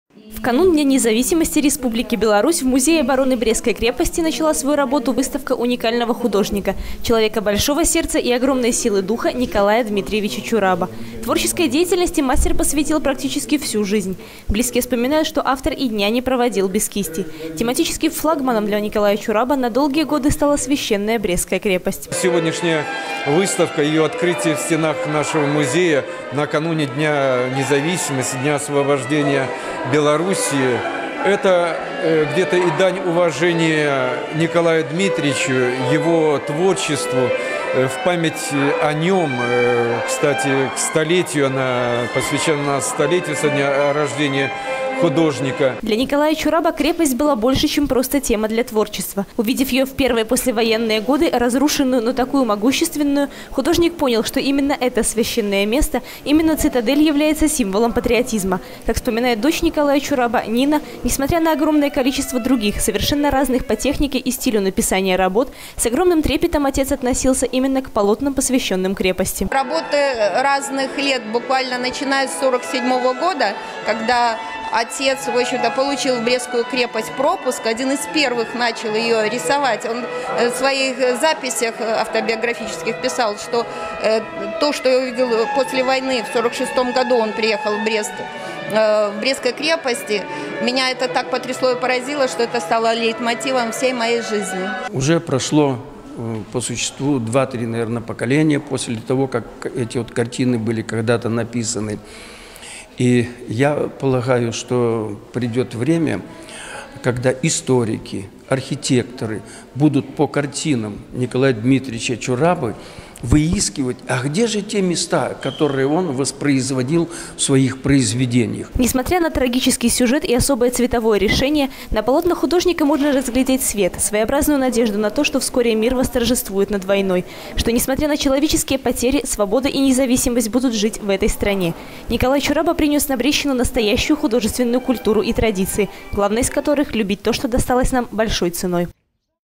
2014-07-03 г. Брест Телекомпания Буг-ТВ. Выставка, посвящённая 100-летию со дня рождения Н. Чурабо
Брестчина богата талантливыми людьми: музыканты, поэты, художники - все они своим творчеством прославляют родной край, рассказывая о его красотках и истории. В этом году свой столетний юбилей отметил бы один из представителей такой творческой интеллигенции Николай Чурабо.
Главная тема работ мастера кисти -- цитадель и подвиг её защитников. 2 июля в Брестской крепости открылась экспозиция Николая Чурабо «Мой Брест - моя крепость». Побывала на торжественном мероприятии и наша съёмочная группа.